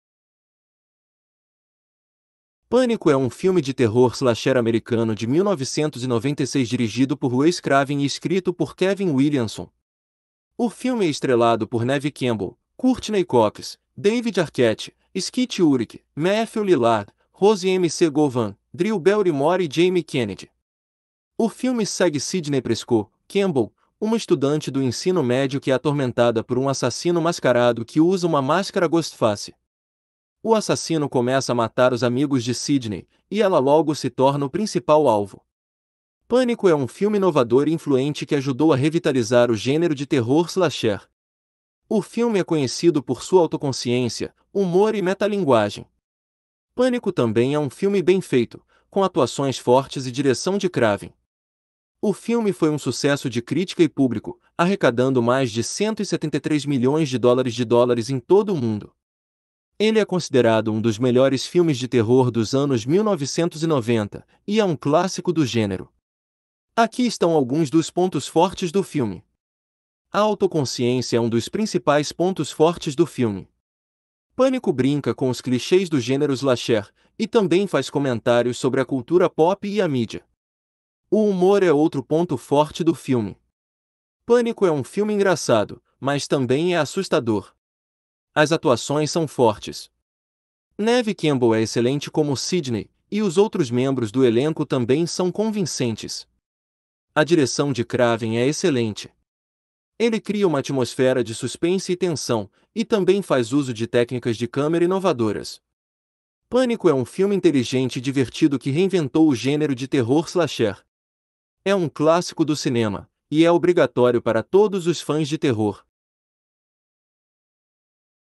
Pânico (1996) - Crítica
Pânico é um filme de terror slasher americano de 1996 dirigido por Wes Craven e escrito por Kevin Williamson. O filme é estrelado por Neve Campbell, Courteney Cox, David Arquette, Skeet Ulrich, Matthew Lillard, Rose McGowan, Drew Barrymore e Jamie Kennedy.